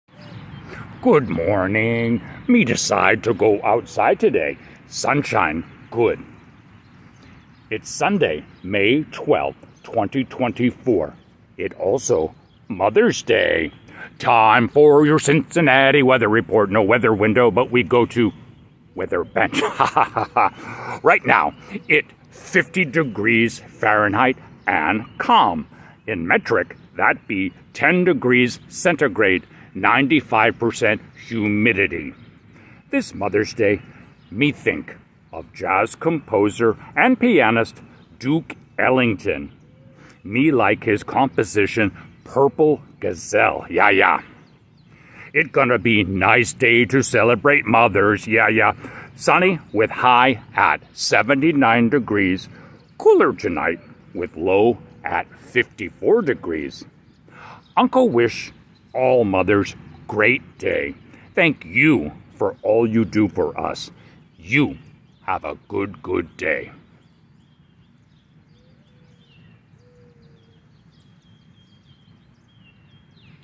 Unka Unka's Weather Report 5/12/25 Sunday Jazz Mother's Day Duke Ellington puppet video
Unka Unka the caveman's Cincinnati weather report for Sunday, May 12, 2024.
Follow Wump Mucket Puppets on social media.